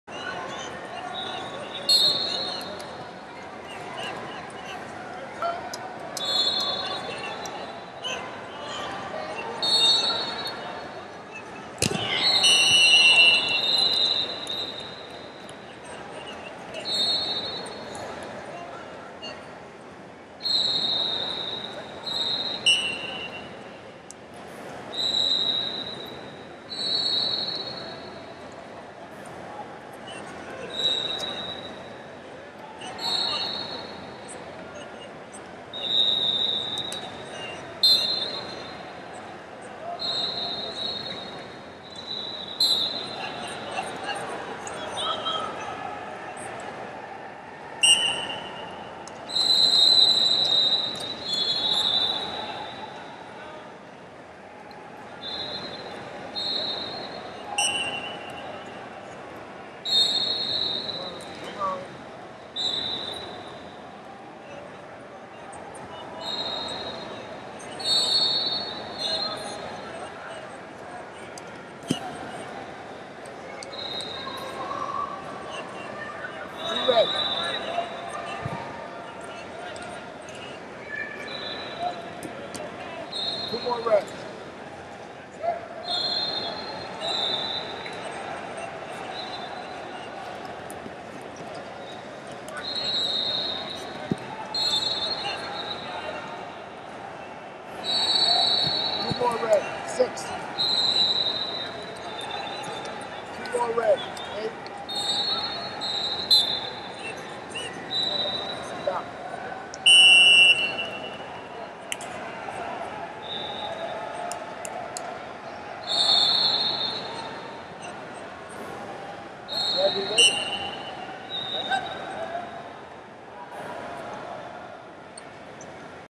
72 Kg Round Of 16 Anthony Artalona PRTC Vs Austen Laughlin Unattached
2019 UWW Cadet And U23 World Team Trials - 72 Kg Round Of 16 Anthony Artalona PRTC Vs Austen Laughlin Unattached